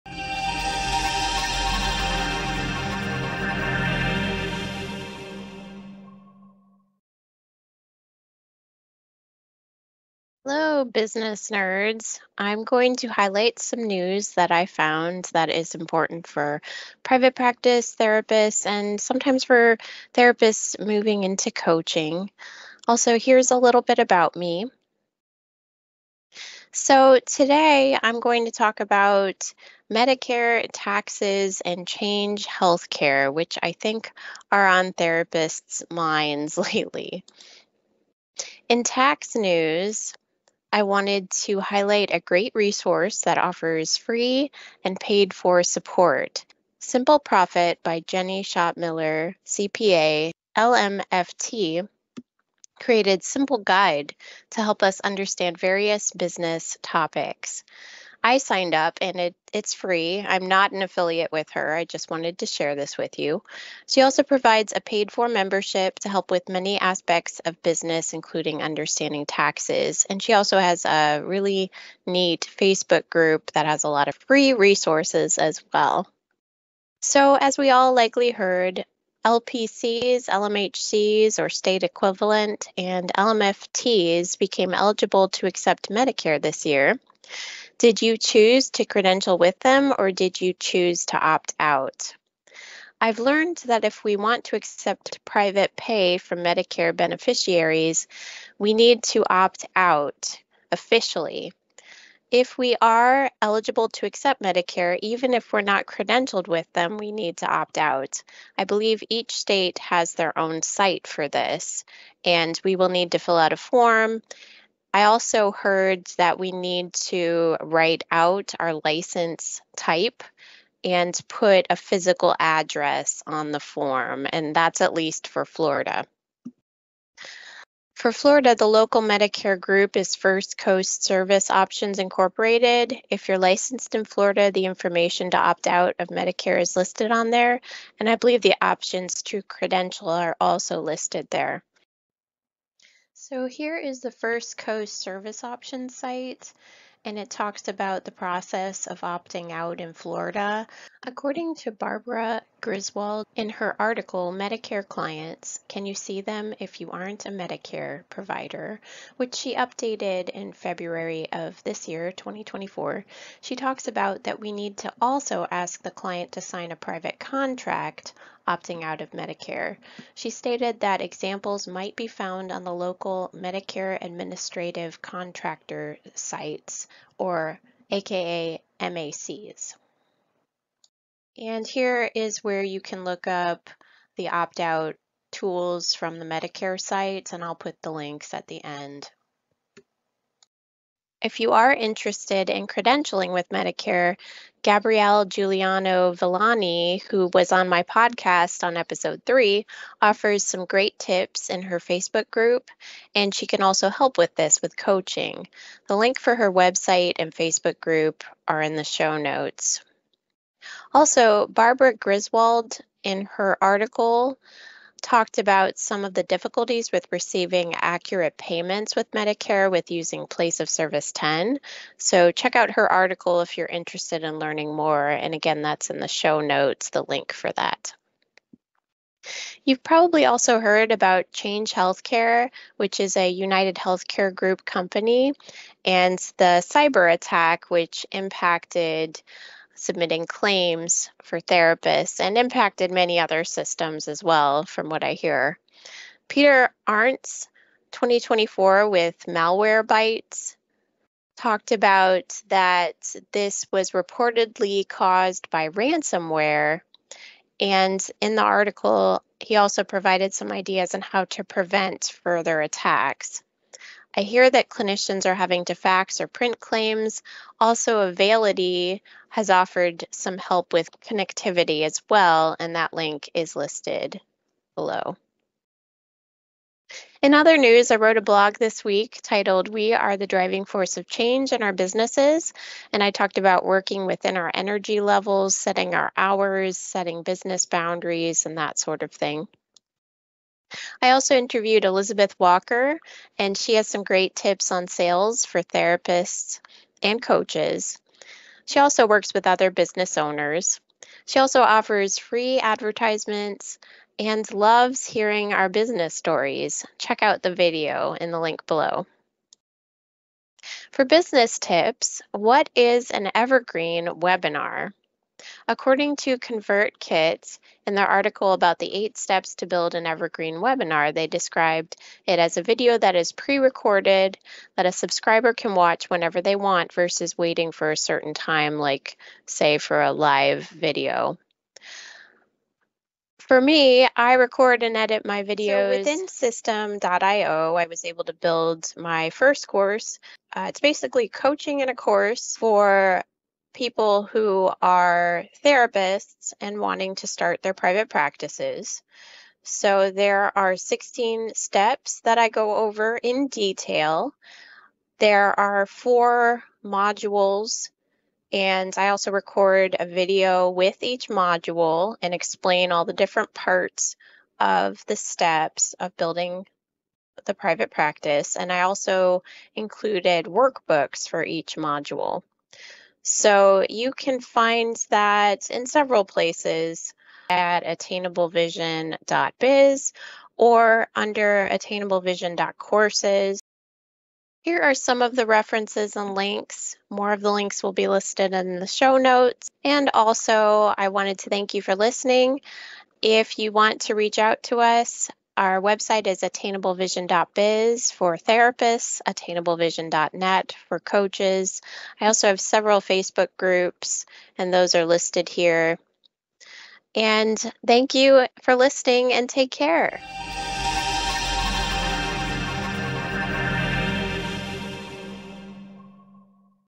Business for Therapist Nerds News: Medicare, Taxes, Change Healthcare
News for therapists about opting out with Medicare, Tax tips and information about Change Healthcare. The copyright should say 2024 for each slide.

Arntz, P. (February 28, 2024). Change Healthcare Outages Reportedly Caused by Ransomware.

ConvertKit (2024). 8 Steps to Create Evergreen Webinars that Generate Leads and Sales on Autopilot.

Griswold, B. (Feb. 2024). IMPORTANT UPDATE FOR ALL MEDICARE PROVIDERS About the Incorrect Payment of 2024 Telehealth Claims (and other claim rejections).
Griswold, B. (updated February 29, 2024b) Medicare Clients: Can You See Them if You Aren’t a Medicare Provider?

Juliano-Villani, G. (2024). Medicare Consulting for Therapists.

Simple Profit (2024). Education and Support for Small Businesses.

Video editing by Brie-Anna Willey